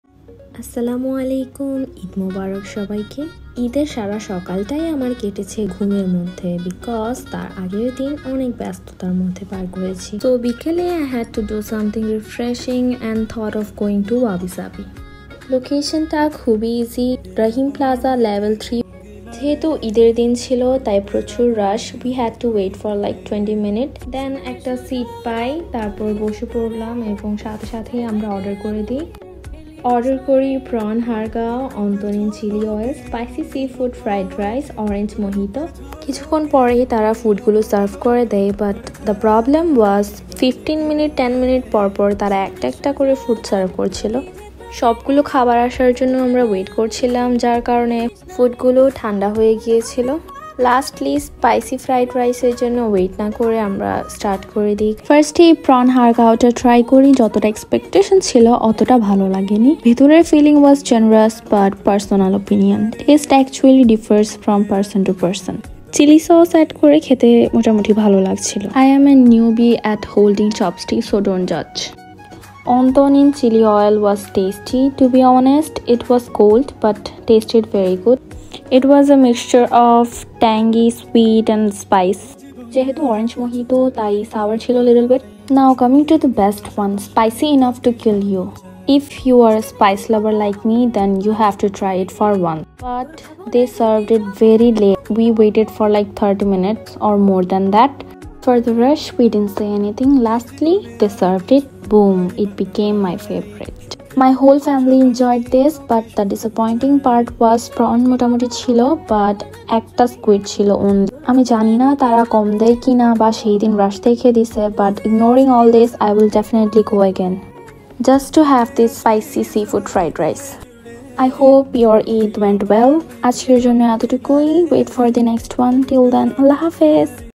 Eid day vlog✨🌙
Location-Wabi Sabi(Rahim plaza ,level 3 khulshi)
1) prawn har gao-365tk
2)wonthon in chilli oil-365tk
3)spicy seafood fried rice-440tk
4)orange mojito-220tk